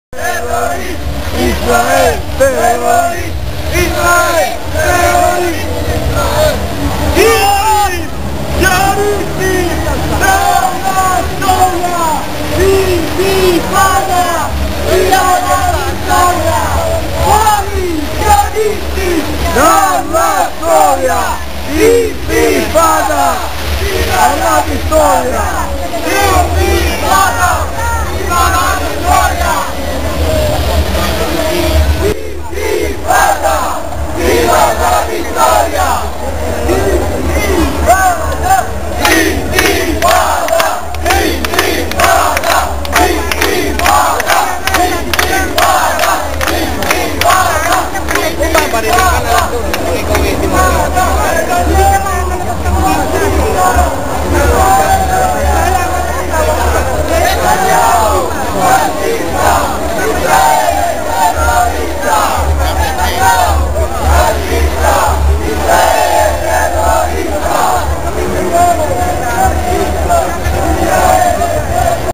FUORI I SIONISTI DALLA STORIA INTIFADA FINO ALLA VITTORIA! .wmv
SABATO 18 GIUGNO 2011 MANIFESTAZIONE NAZIONALE CONTRO L'OCCUPAZIONE NAZISIONISTA DI MILANO
NETANYAU FASCISTA ISRAELE TERRORISTA ! INTIFADA FINO ALLA VITTORIA!